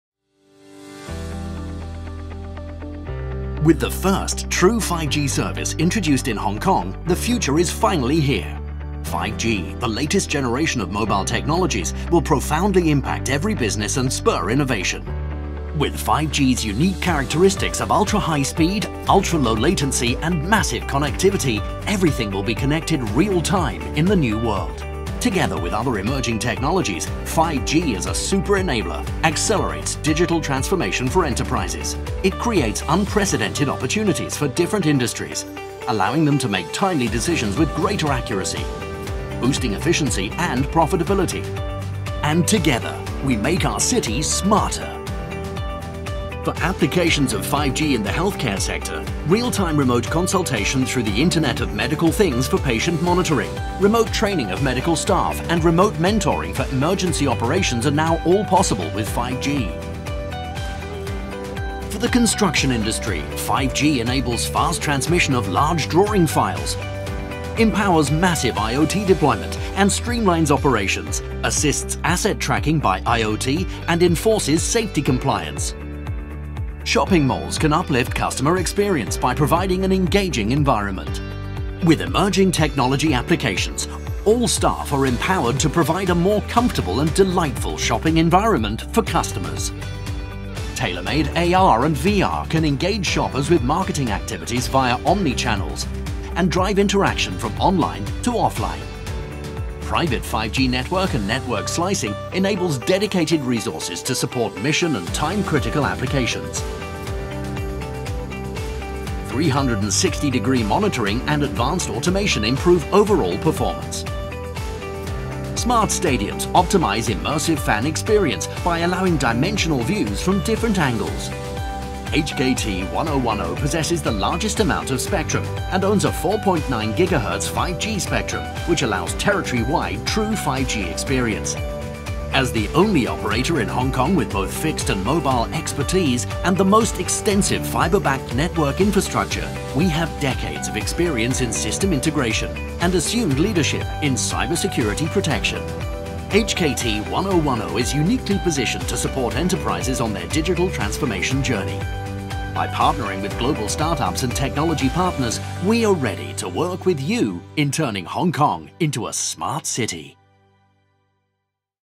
5G for business
HKT & 1O1O owns the most 5G/4G/3G spectrum in Hong Kong.  Together with our proven capability in delivering end-to-end integrated solutions using the above emerging technologies, we empower digital transformation for enterprises.